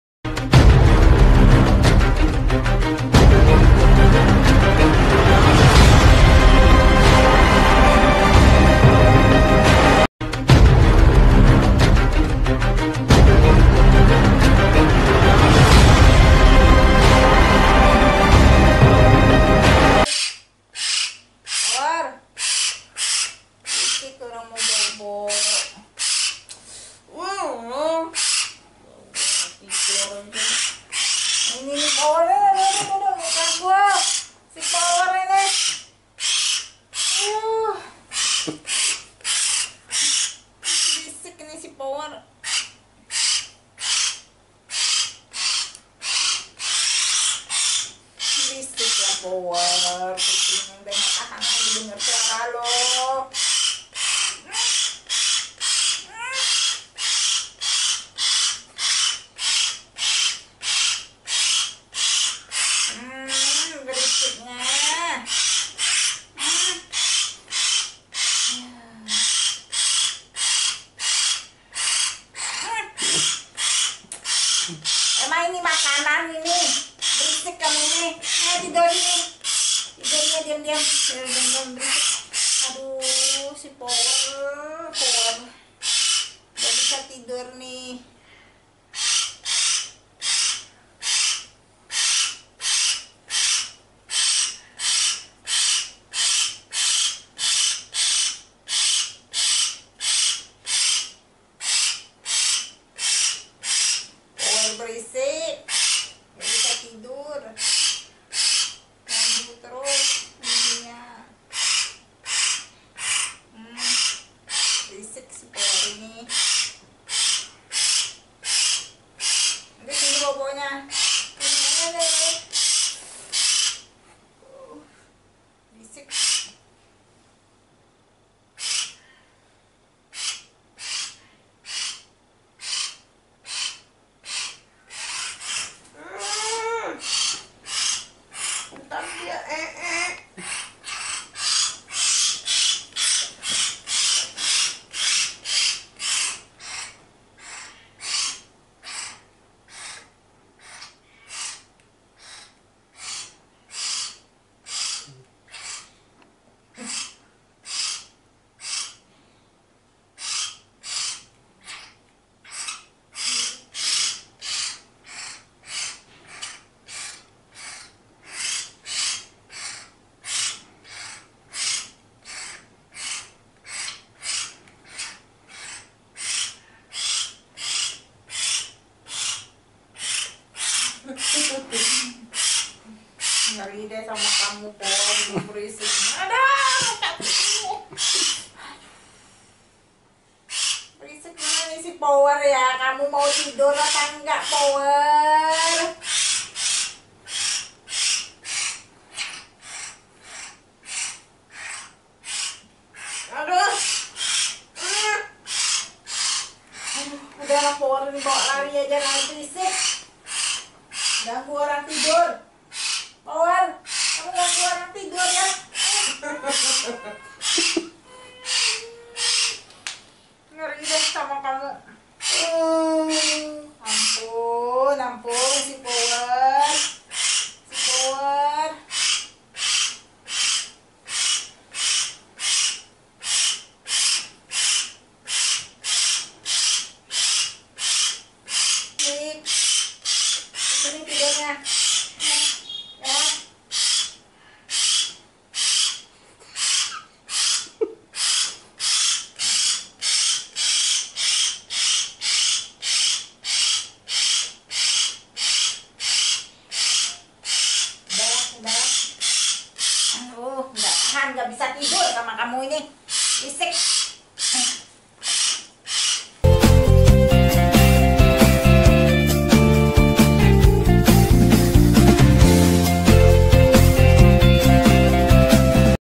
2 Cewek Cantik ini Coba Tidur Dengan Seekor Anak Beo, Beo Lucu Pintar
Bagaimana reaksi manusia tidur bersama dengan seekor anak Beo..
Video ini memberian wawasan dan hiburan..
Selamat Menyaksikan..